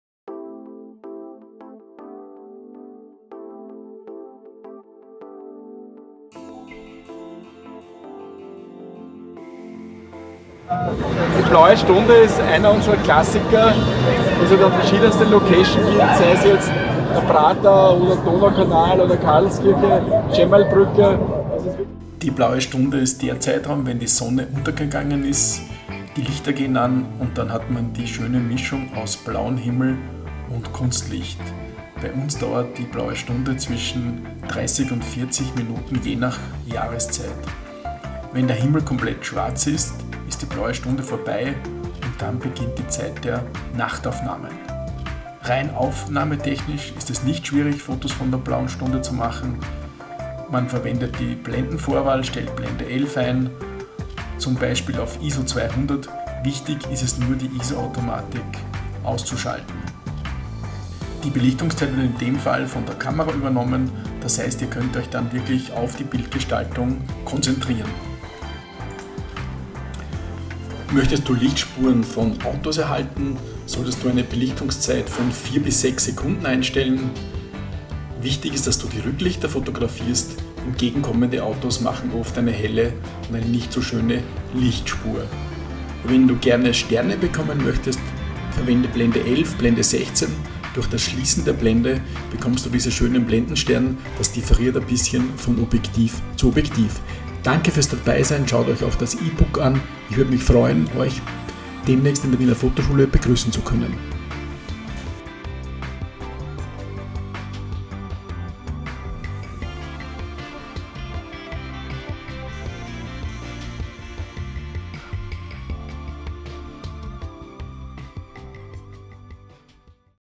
Teaser zu den Blaue Stunde Kursen der Wiener Fotoschule
LEITFADEN BLAUE STUNDE - KARL FÜSSELBERGER
In der blauen Stunde gelingen faszinierende Bilder. Karl erklärt Dir wie Du zu außergewöhnlichen Blaue Stunde Bilder kommst.

Der Begriff „Blaue Stunde“ bezieht sich auf die besondere Färbung des Himmels während der Zeit der Dämmerung nach Sonnenuntergang und vor Eintritt der nächtlichen Dunkelheit. Besonders geprägt wurde der Begriff von Schriftstellern und Dichtern, die ihn häufig mit melancholischen Gefühlen assoziieren. Dieselbe Färbung ist auch während der Morgendämmerung zu sehen, allerdings wird der Begriff in diesem Zusammenhang seltener verwendet. Das Blau des Himmels hat eine andere spektrale Zusammensetzung, da es auf eine andere physikalische Ursache als bei Tage zurückzuführen ist. Während der blauen Stunde besitzt dieser tiefblaue Himmel etwa dieselbe Helligkeit wie das künstliche Licht von Gebäude- und Straßenbeleuchtungen.  Gelb und Blau sind auch Komplementärfarben, die uns Menschen besonders ansprechen.
Infos zu den Seminaren findest auf der Webseite der Wiener Fotoscchule
Hier kannst Du auch ein ebook zum Thema Blaue Stunde herunterladen:  t.ly/LBeO

Nächster Fotokurs Blaue Stunde:
Mo, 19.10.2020, 16:30 -19:30 Uhr, Urania Wien: t.ly/h7mN